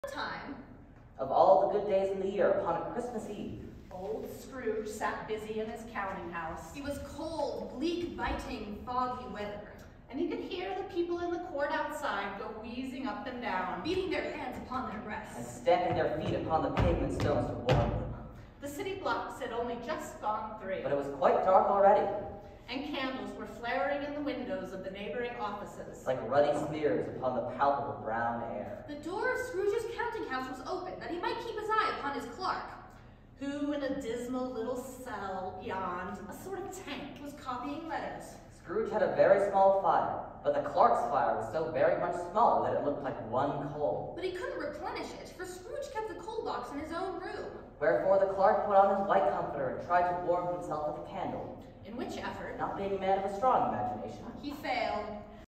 Cast Gin Punch reads from Stave 1 of Charles Dickens' A Christmas Carol
Quinn Nelson - Reader 1
Megan McClean - Reader 2
Taylor Nelson - Reader 3